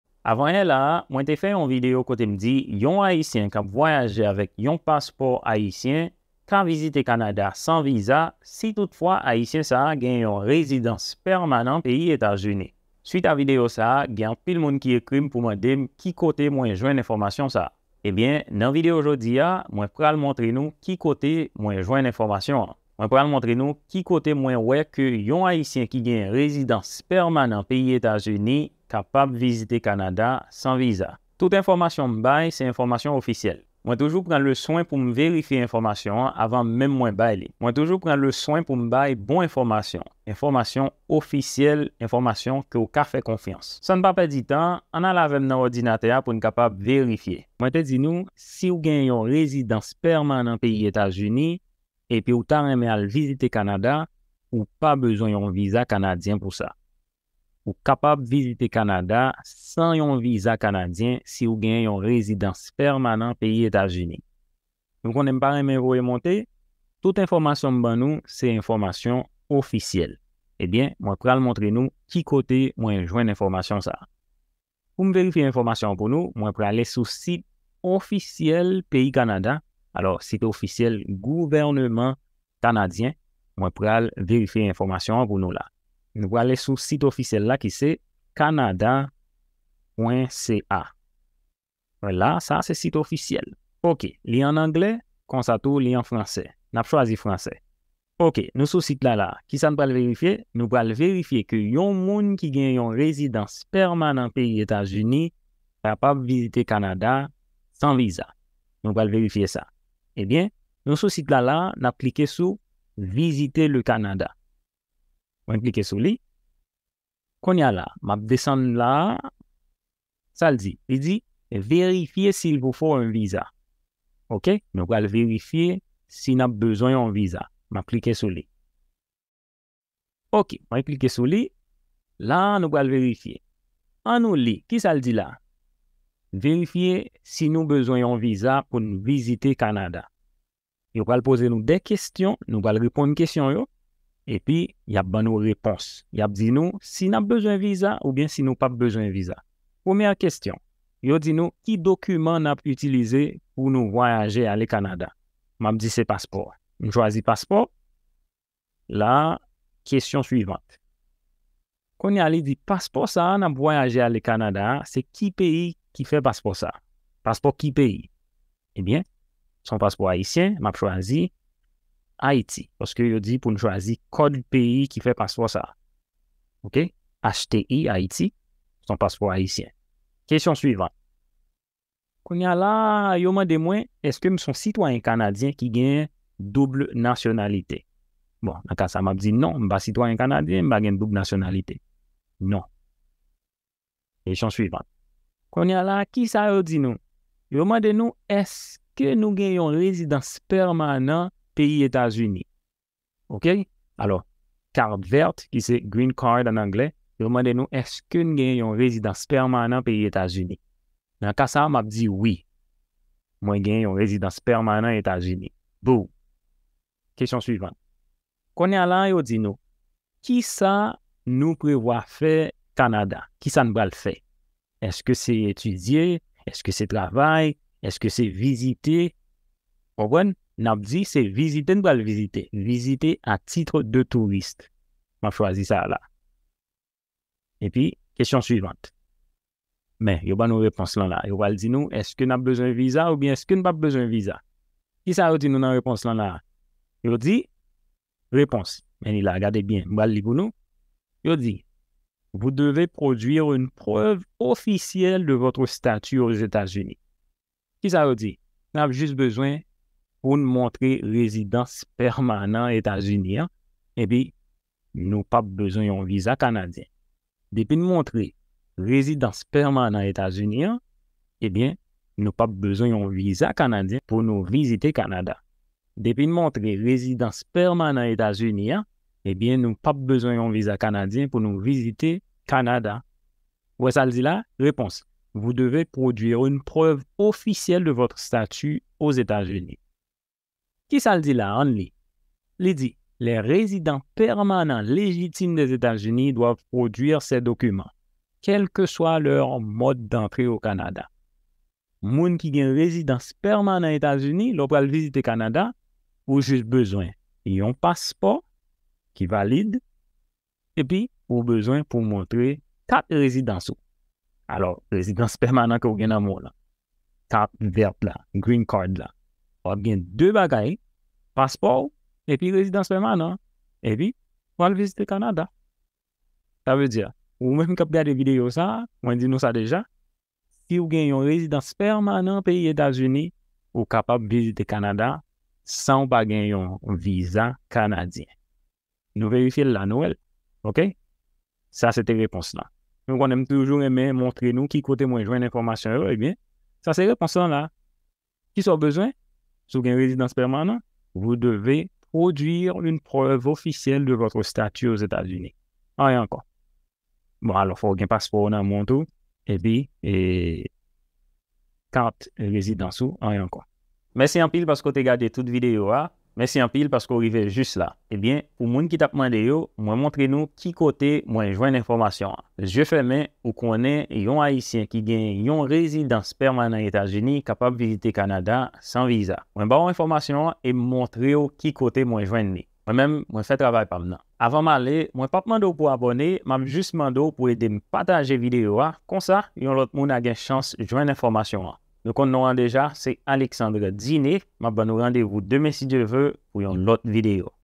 VIZITE CANADA SAN VIZA NAN FASON SA
SA se fason yon ayisyen ka vizite Canada san viza.

NB: Mwen pa konnen aprè konbyen tan wap gen chans gade video sa a. Mwen vlew mete nan tèt ou ke enfòmasyon mwen bay yo ka chanje kenpòt lè. Mwen baw konsèy pouw toujou verifye avan menm ou achte tikèw.